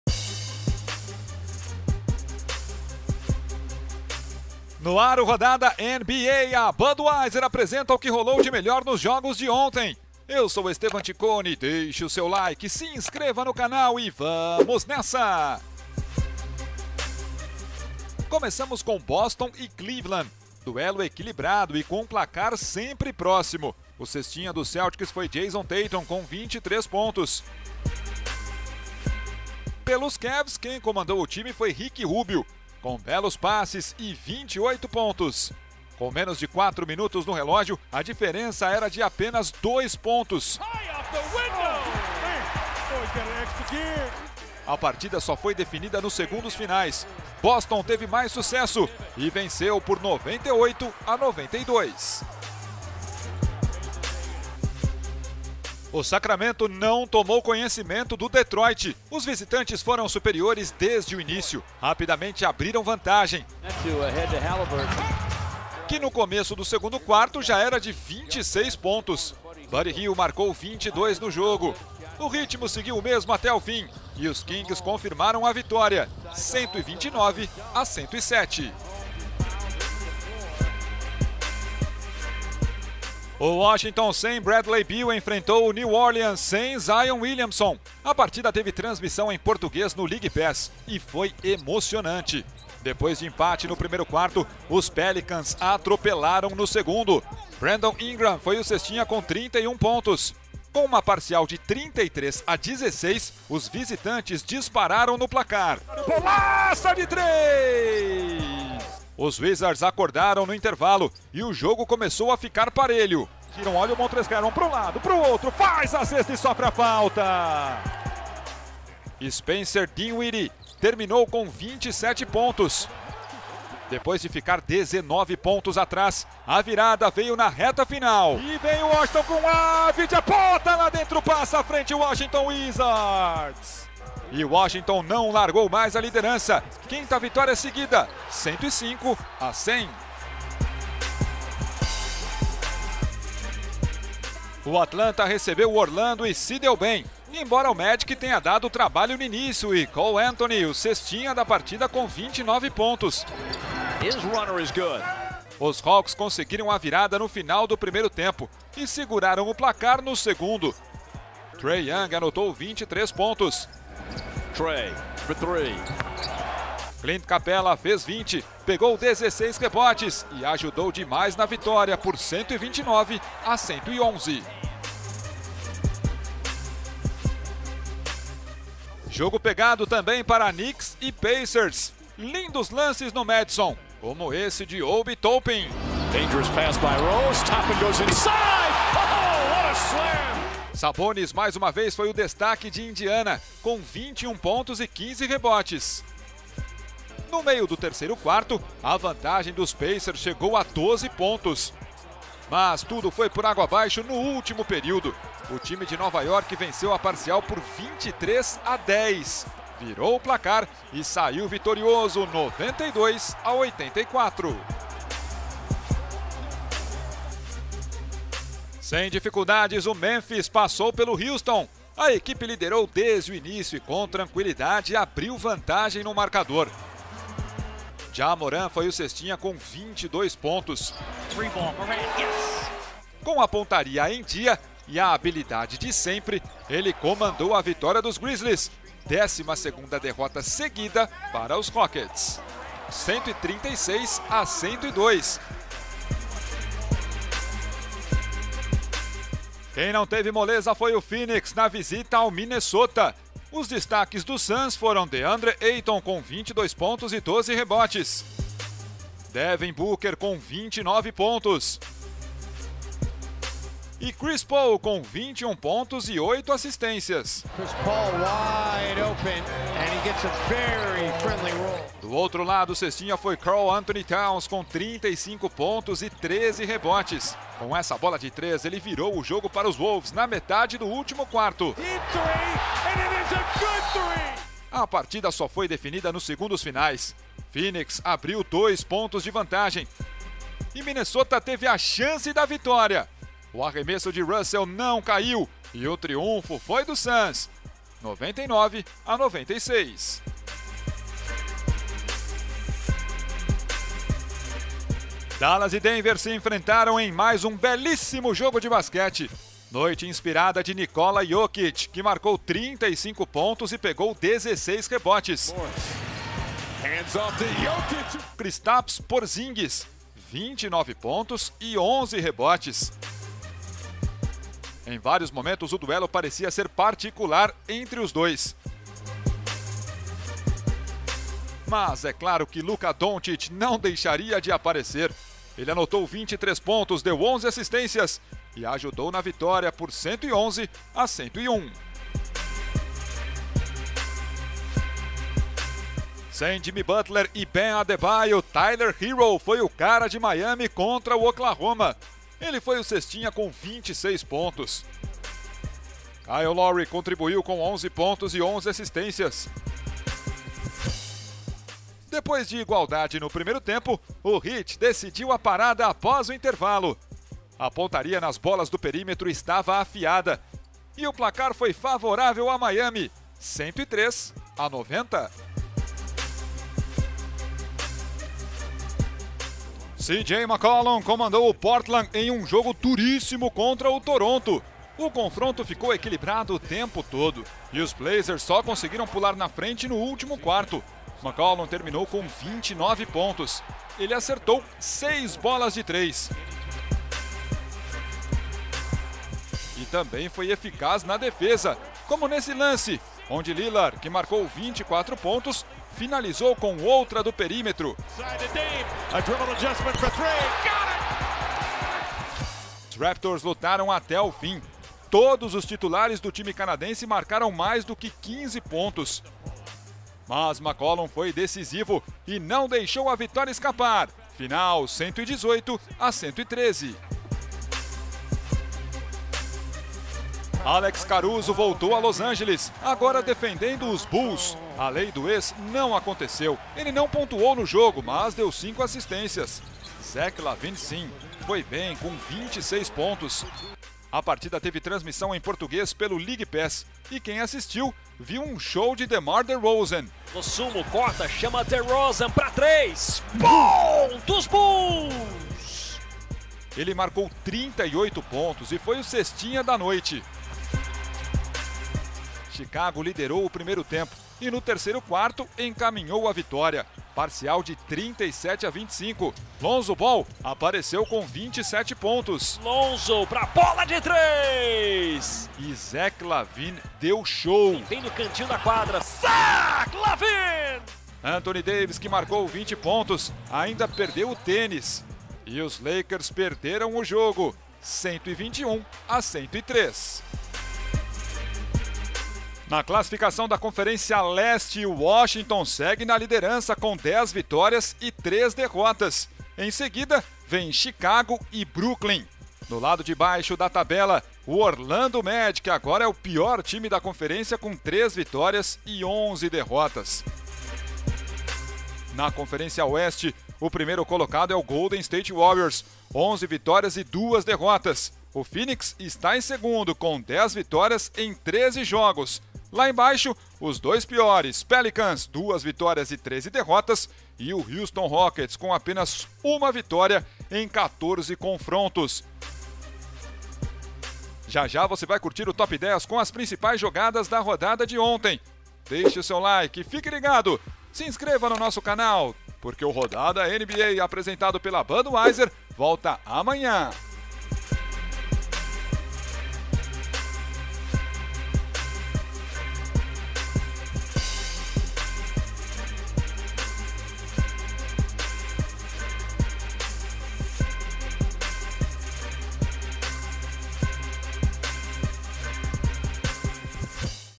JOGÃO ENTRE BULLS E LAKERS, DONCIC X JOKIC E MUITO MAIS - RODADA NBA 15/11
Rodada de segunda com show de DeMar DeRozan no jogo entre Bulls e Lakers. Com 38 pontos, ele garantiu a vitória para o Chicago. Dia também de duelo entre Nikola Jokic e Luka Doncic. Com a ajuda de Porzingis, Luka levou o Dallas à vitória. Teve ainda: Portland passando pelos Raptors, Phoenix ganhando a nona seguida e muito mais!

O Rodada NBA vai ao ar TODOS OS DIAS trazendo um resumo de tudo o que rolou nos jogos da noite anterior.